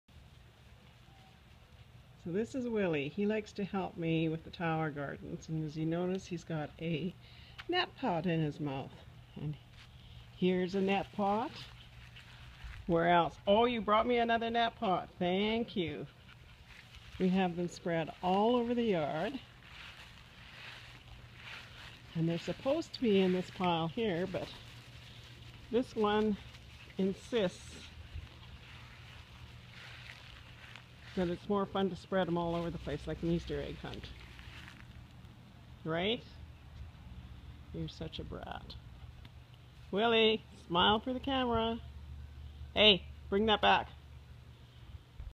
Willy The Maltese Helping With Tower Garden Maintenance
Every 60 days I like to take my Tower Garden apart to trim roots and clean the Tower itself. My Maltese Willy loves to help. He is so helpful. :)